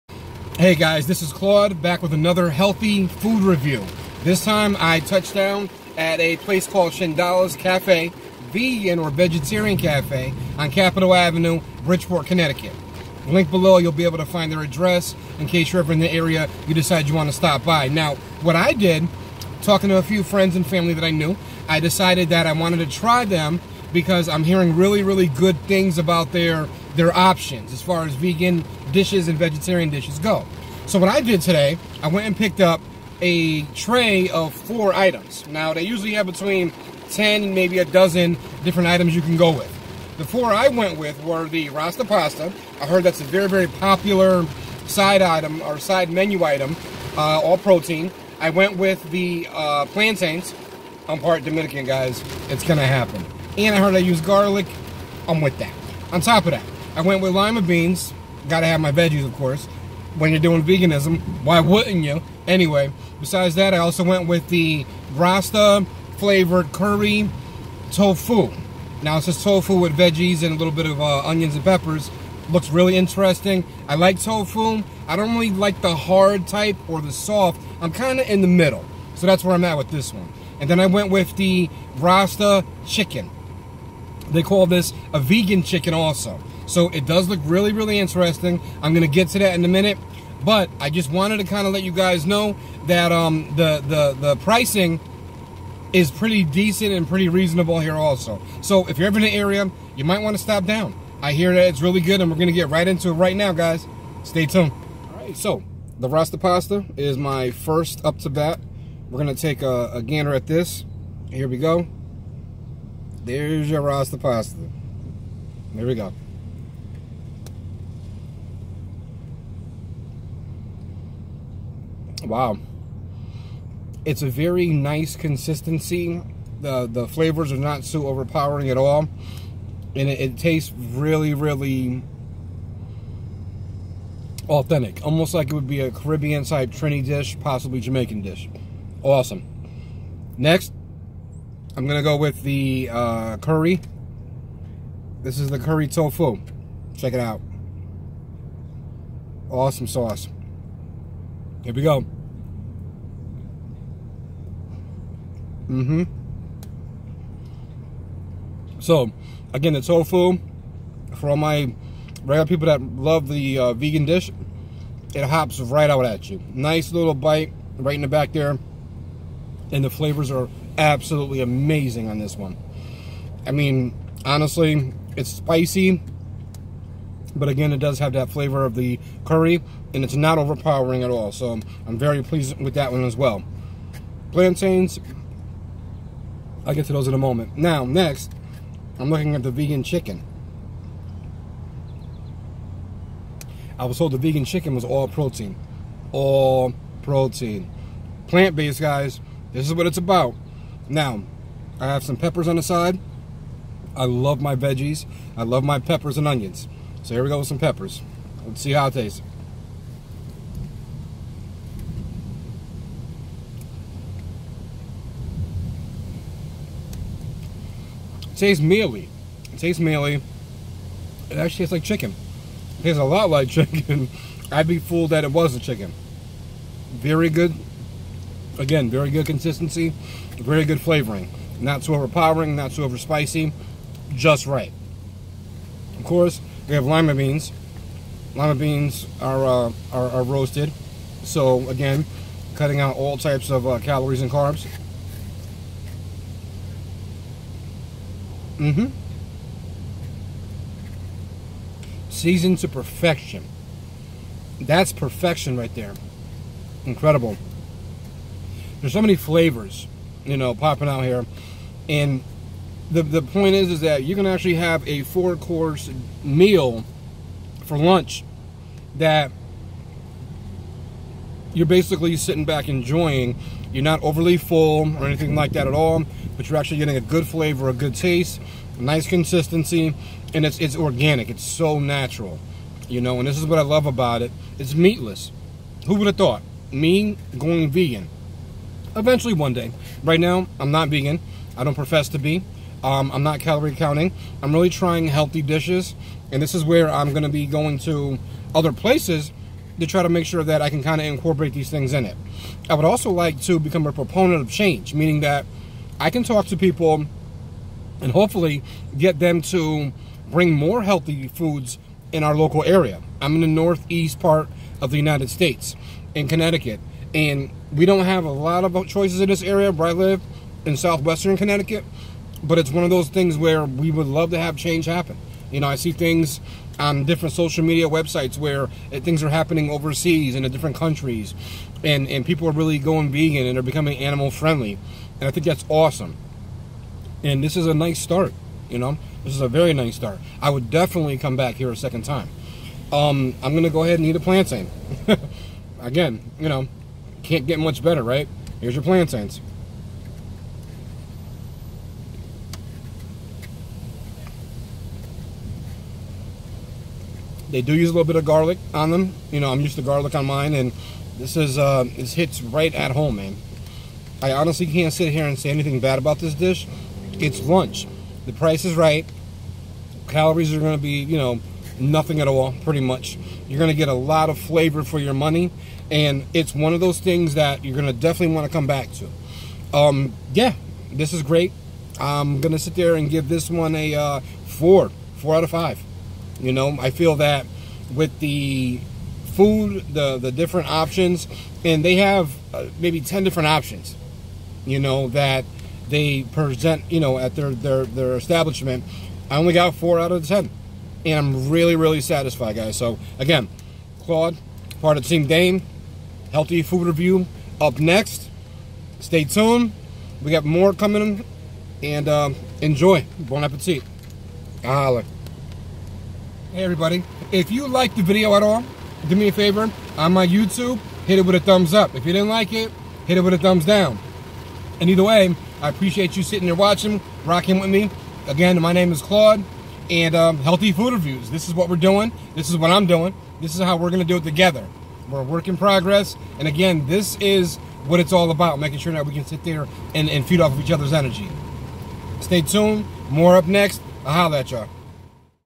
SHANDALS VEGETARIAN CAFE
So I stopped by Shandals Vegetarian Cafe, in Bridgeport, CT, located at 520 Capital Avenue. Great experience overall...I would try more of this delicious food again!